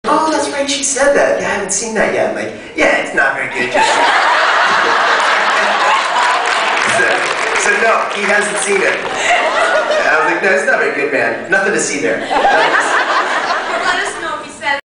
Rob talking about Still Waiting
He just said he had to makeout with Danneel multiple times and it wasn't too hard since she's hot and then he books Supernatural and meets Jensen.